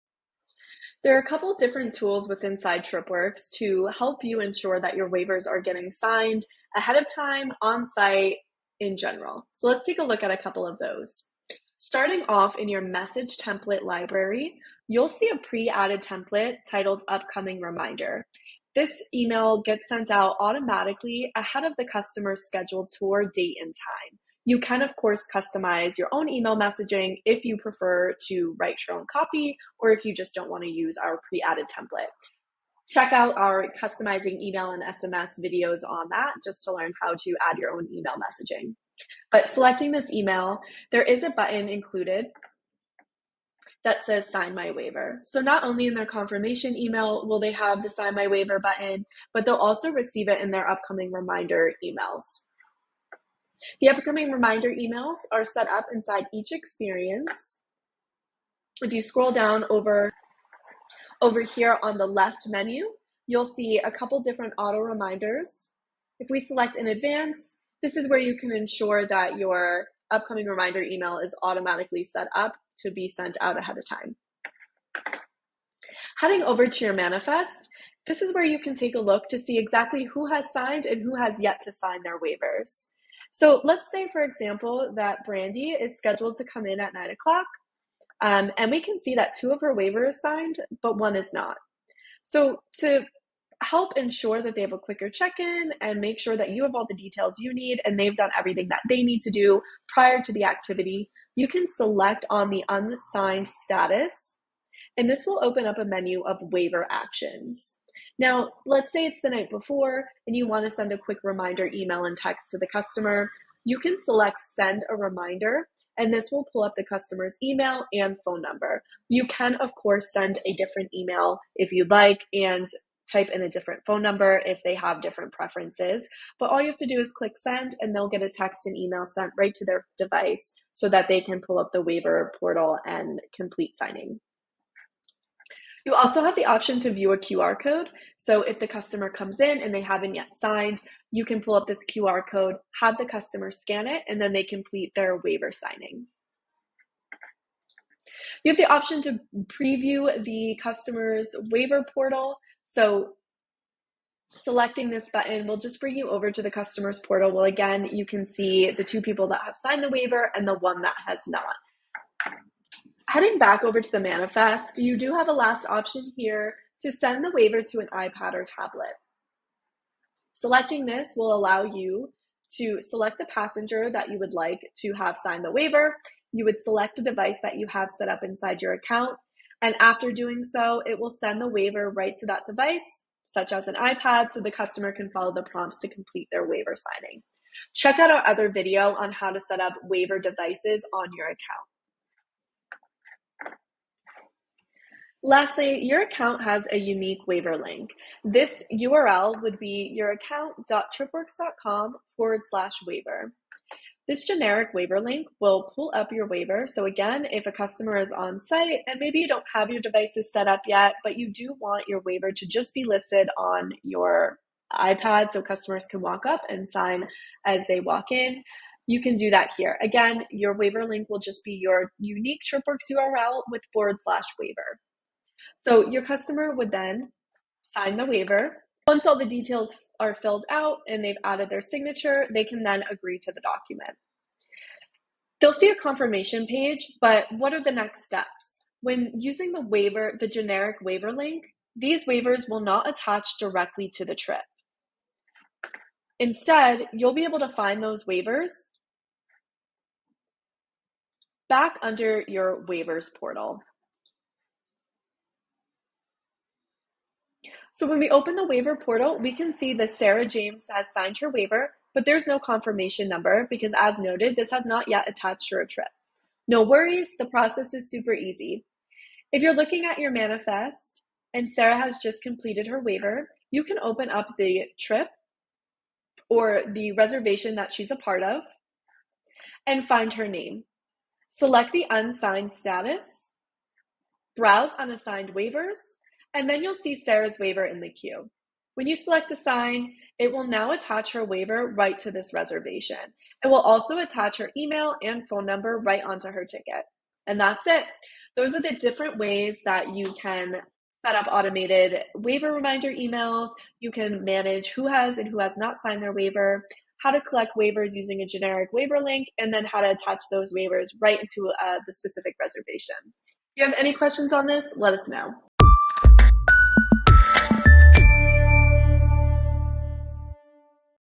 Setting Up Waiver Reminders, Links, and the Portal
This video explains various tools in TripWorks for ensuring customers sign their waivers on time. It covers how to set up automated reminder emails with a "Sign My Waiver" button, view waiver statuses in the manifest, and send text or email reminders. Additional options include generating a QR code for in-person waiver signing, previewing a customer's waiver portal, or sending waivers directly to a tablet. The video also shows how to use a generic waiver link for on-site signing and how to manually attach unsigned waivers to reservations.

0:00 - 0:19: Introduction to tools for ensuring waivers are signed ahead of time.
0:19 - 0:45: Overview of pre-added and customizable waiver reminder emails.
0:45 - 1:59: Managing waiver status via the manifest, sending reminders, and using text/email options.
1:59 - 3:06: Utilizing QR codes, waiver portal previews, and sending waivers to a device like an iPad.
3:06 - 4:53: Using a unique waiver link and assigning waivers to reservations.
4:53 - 5:21: Conclusion on automated waiver management and attachment.